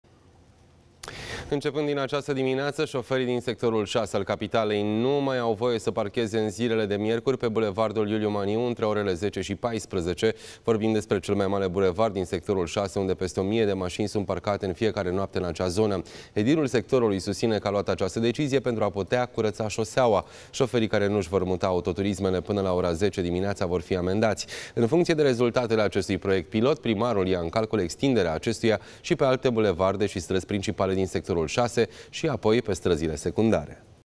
Parcarea interzisă pe bulevardul Iuliu Maniu din Bucureşti, în fiecare zi de miercuri
Începând din această, şoferii din Sectorul 6 al Capitalei nu mai au voie să parcheze pe bulevardul Iuliu Maniu în zilele de miercuri, între orele 10.00 şi 14.00. Vorbim despre cel mai mare bulevard din Sectorul 6, unde peste o mie de maşini sunt parcate în fiecare noapte. Primarul susţine că a luat această decizie pentru ca bulevardul să fie curăţat.